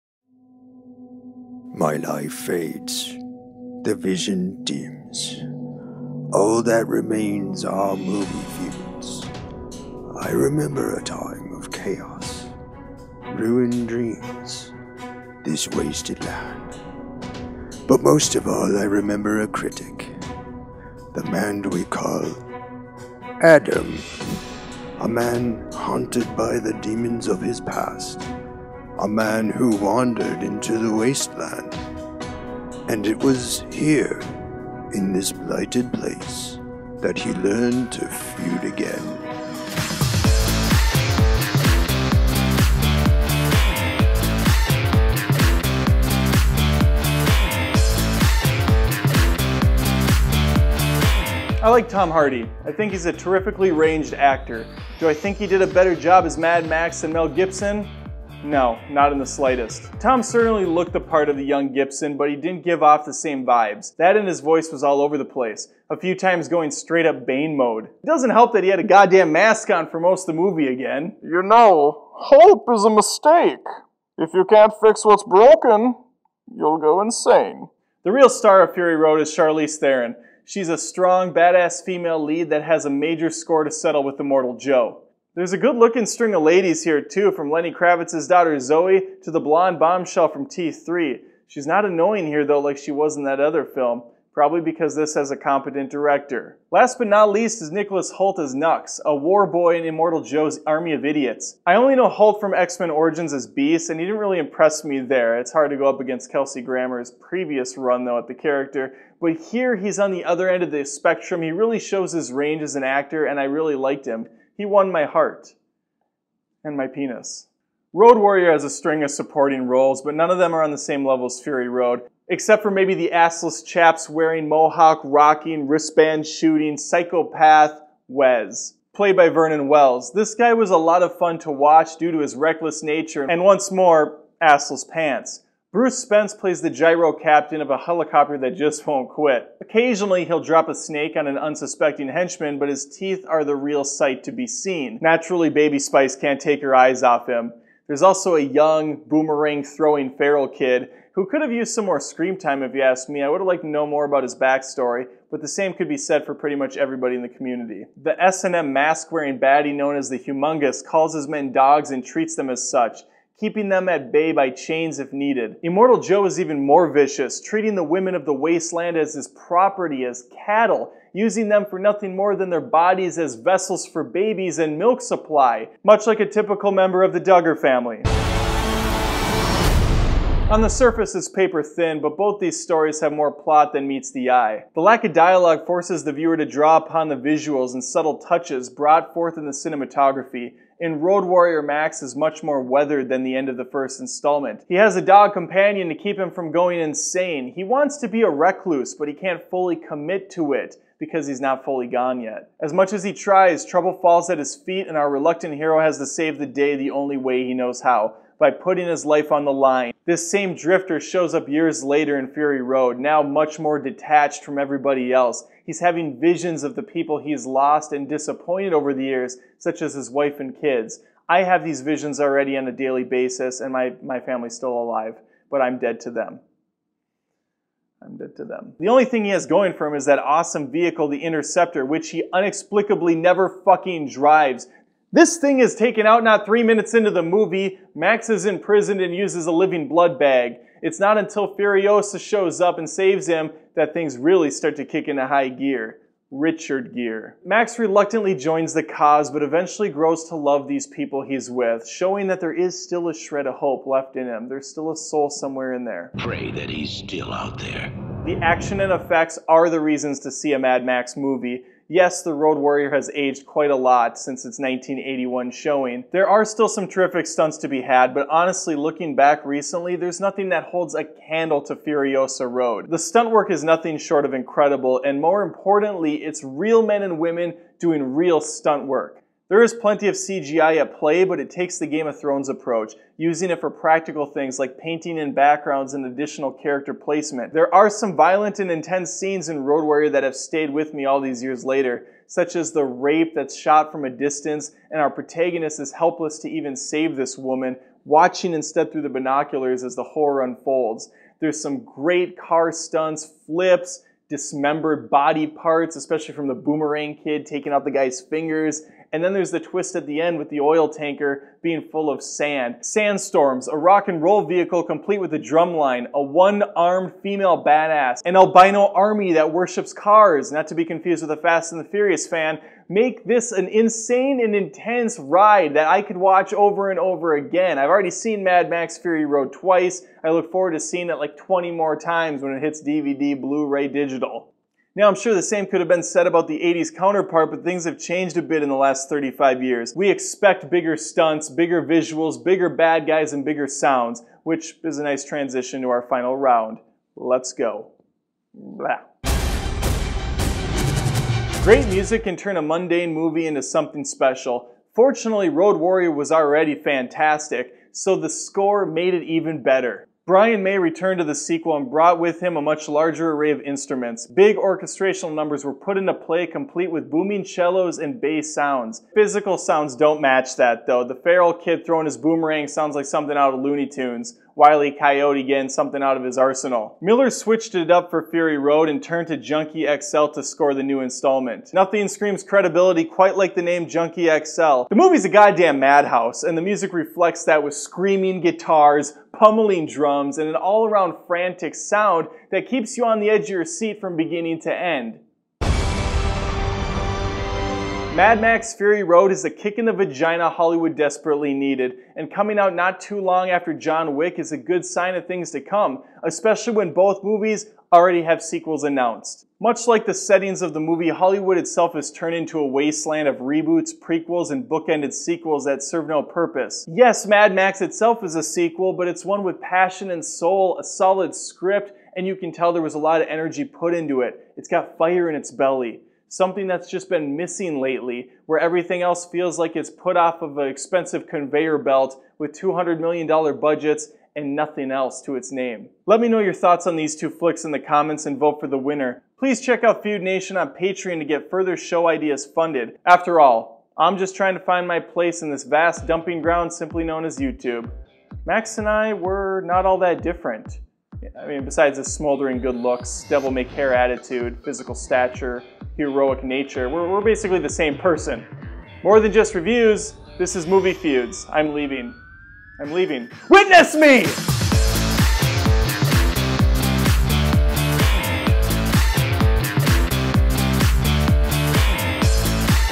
Mad Max 2: The Road Warrior VS Mad Max: Fury Road : Movie Feuds ep140
Mad Max: The Road Warrior has held up tremendously well. When comparing it to Fury Road however, that's another story.
Adam takes a look at the pros and cons of both films; like the leads Mel Gibson and Tom Hardy, the supporting characters, the action and effects and the overall plot. What a lovely day for a feud!